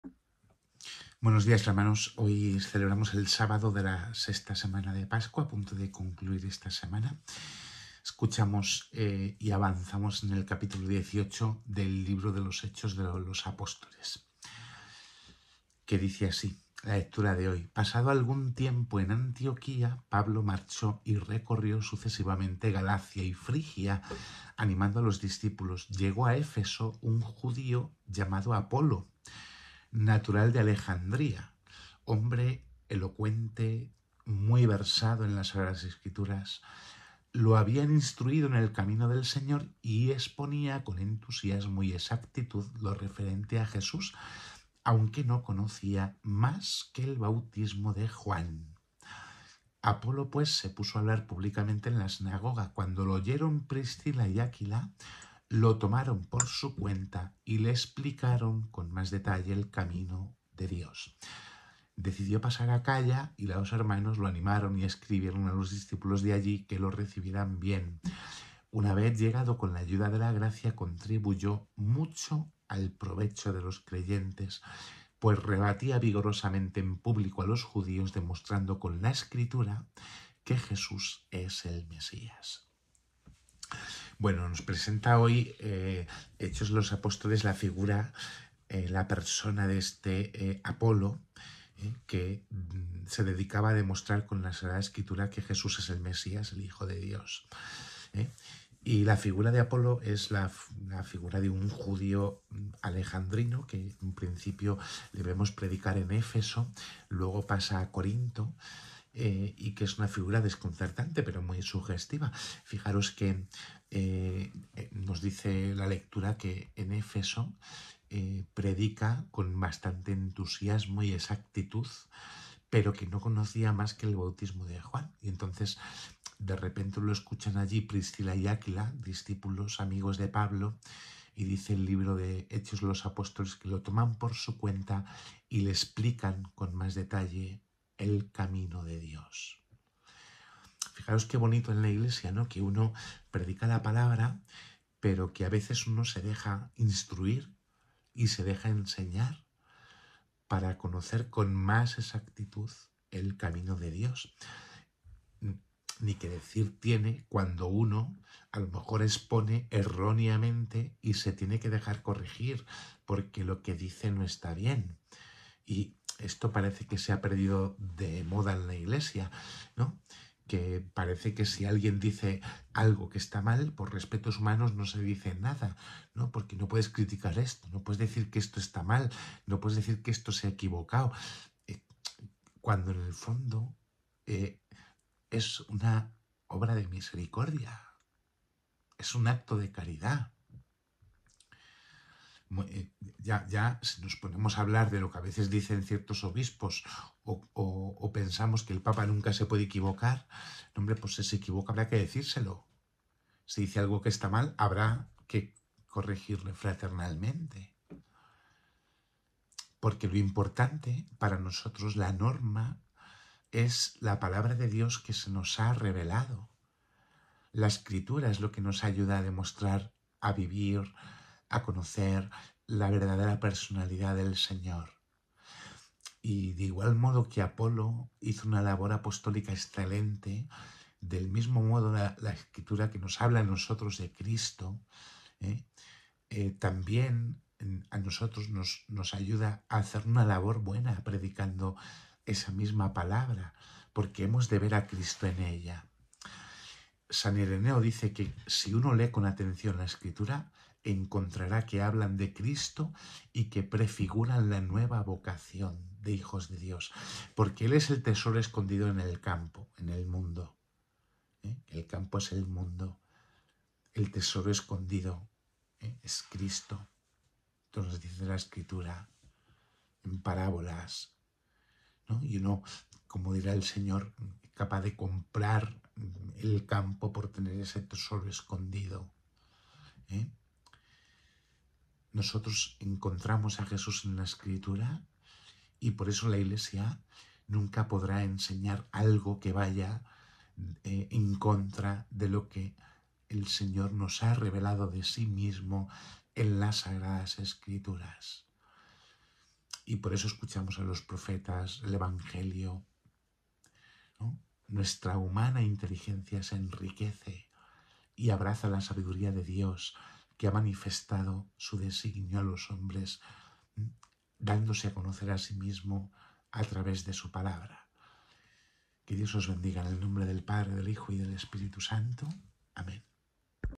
Comentario al Evangelio del día 20 de mayo
Comentario a la Primera lectura (Hechos de los Apóstoles) del día 20 de mayo, sábado de la sexta semana de Pascua